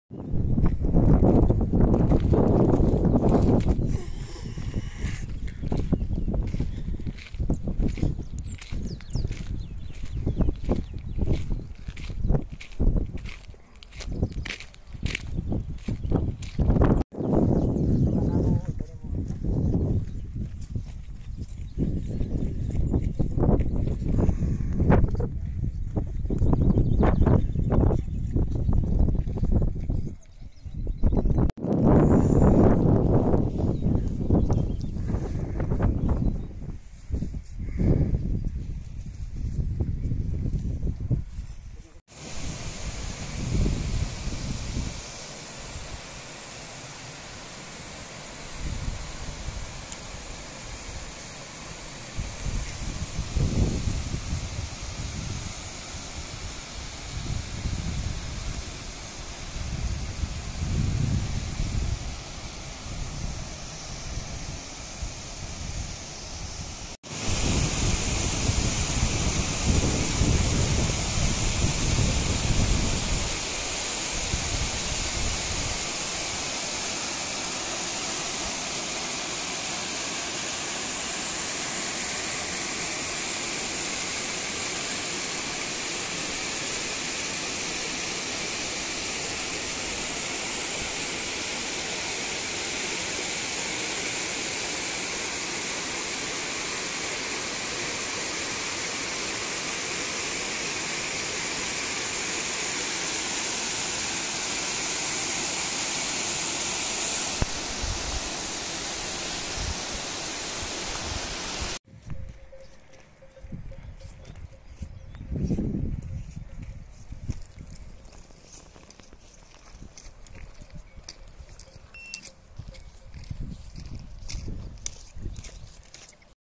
Cascada El Hornillo Puerto Malagón . Senderismo Comunidad de Madrid
Caminaremos entre la divisioria de la Comunidad de Madrid y la Comunidad de Castilla y León por su parte superior de nuestra ruta, y el Sitio Natural de Interés Nacional Pinar de Abantos y zona de la Herrería del Real Sitio de San Lorenzo del Escorial.

Por caminos fácilmente transitables obtendremos unas magníficas vistas que contemplar, adentrandonos en pinares y disfrutando del discurrir de las aguas entre sus arroyos y embalses, colmados de líquido elemento en esta época.

La población de Robledondo,pertenece al partido de Santa María de la Alameda.